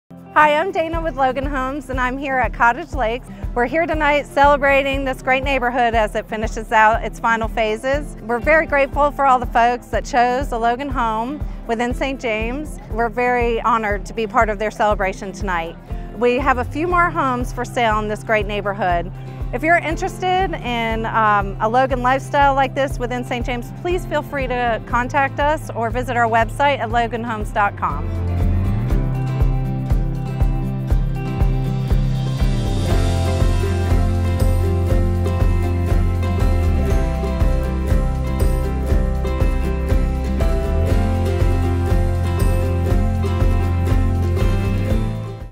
Cottage Lakes Block Party
We had a wonderful time with Logan homeowners for the first Cottage Lakes Block party as we celebrate the near-completion of this neighborhood! We started this St James Plantation neighborhood in 2015 and we have loved seeing it grow. Now, we have had a chance to see all the neighbors are doing to help create even more of a sense of community, and a place to belong. If this sounds like a place you'd like to call home - we still have a couple homes left!